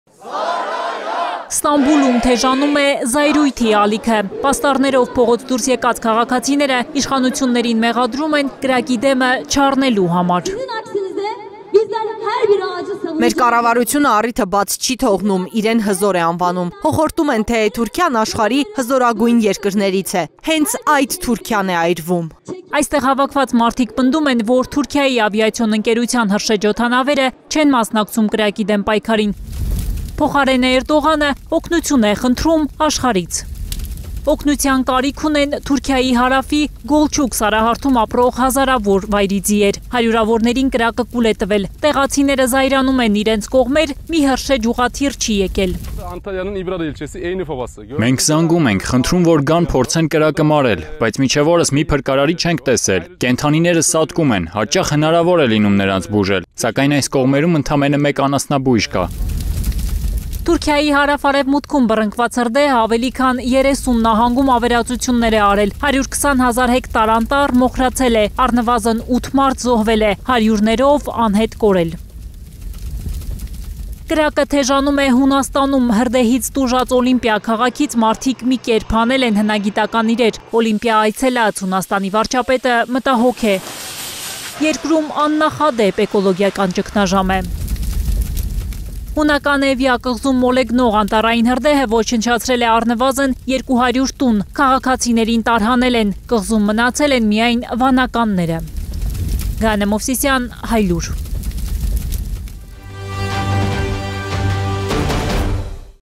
«ԹՈՒՐՔԻԱՆ ԱՅՐՎՈՒՄ Է». ԿՐԱԿՈՏ ԲՈՂՈՔ ՍՏԱՄԲՈՒԼՈՒՄ ԻՇԽԱՆՈՒԹՅԱՆ ԴԵՄ
«Թուրքիան աշխարհի հզորագույն երկրների՞ց է: Բա ինչու՞ է այրվում»: Զայրացկոտ հարցերով թուրքերը փողոց են դուրս եկել բողոքի գործողության: Հիմա իշխանությունը չգիտի կրա՞կը զսպել, թե՞ թեժացող ակցիաները: Անճարությունից Էրդողանը հայտարարել է, որ  250 միլիոն ծառ կտնկի հրդեհներից տուժած շրջաններում: